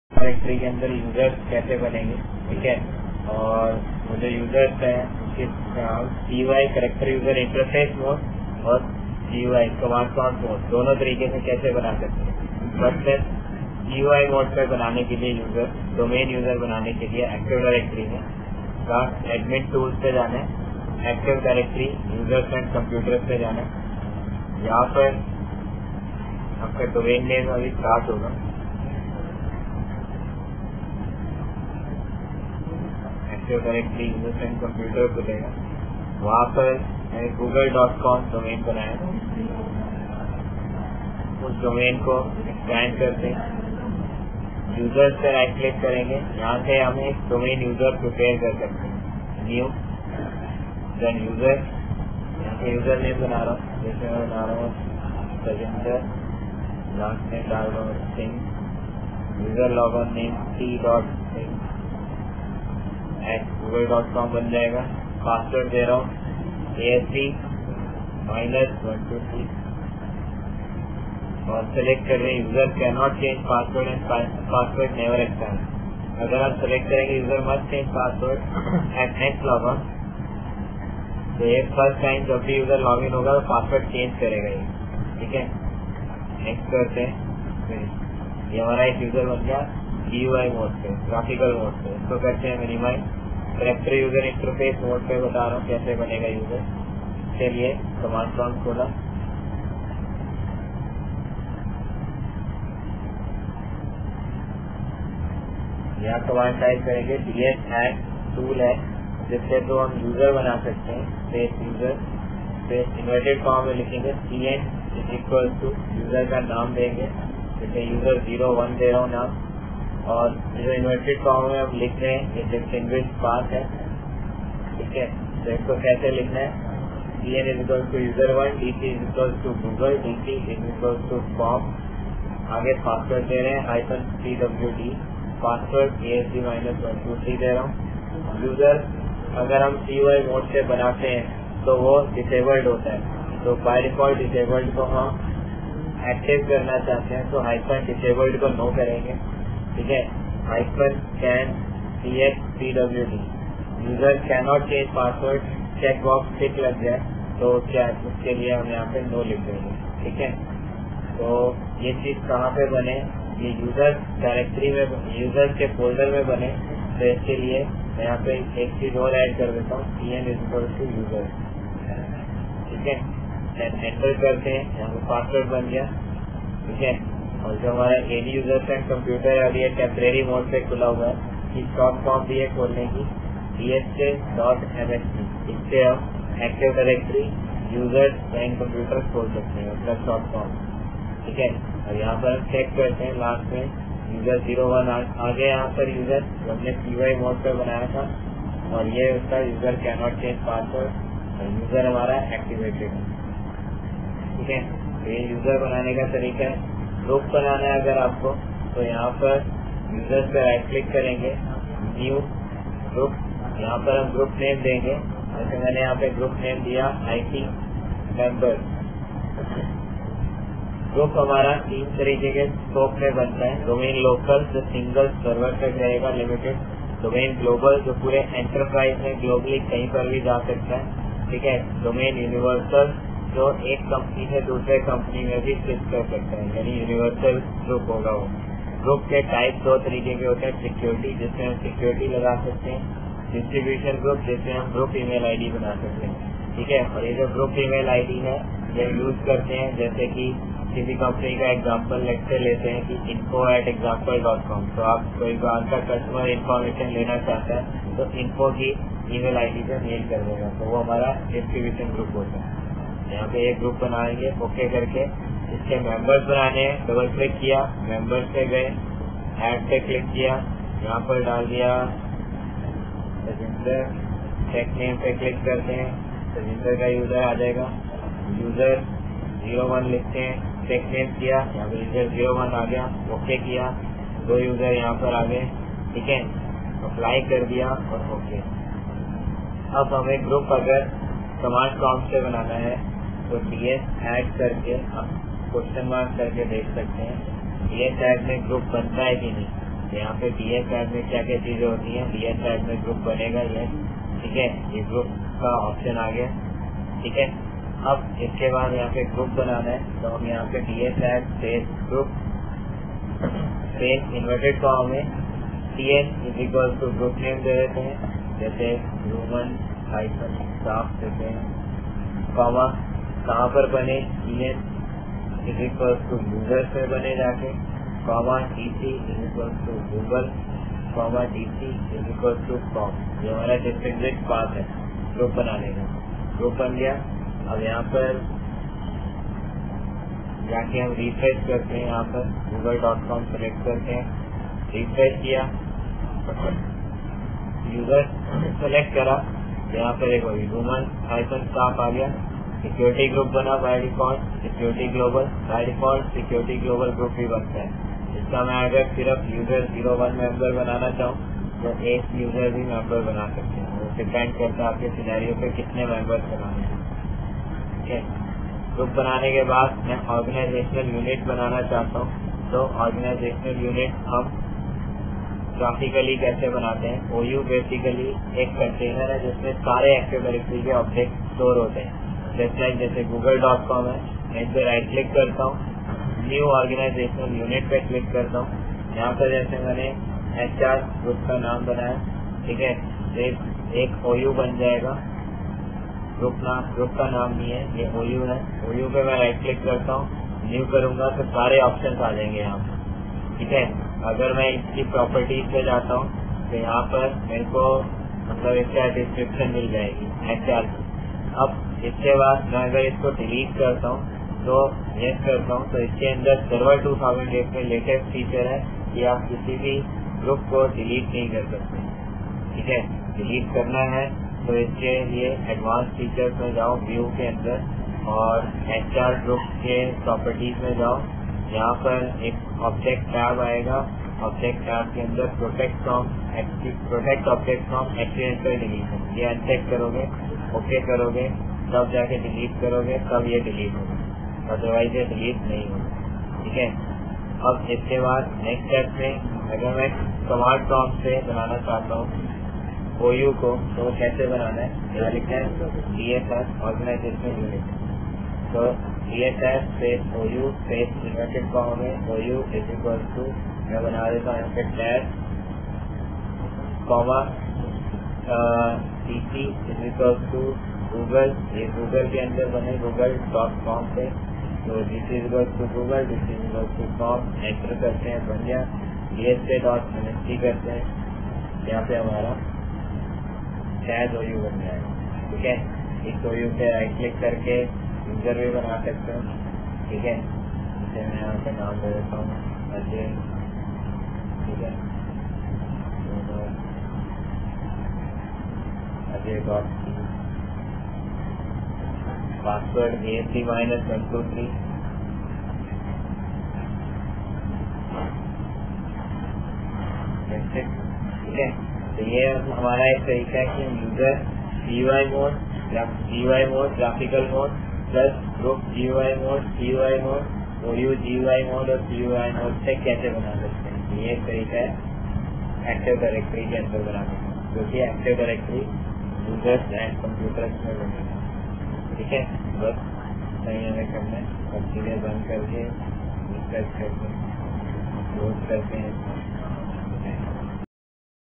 AD Users, groups and Organizational Unit
AD Users, groups and Organizational Unit for windows 2008 AD Server developed by Tajinder Singh for MCITP students.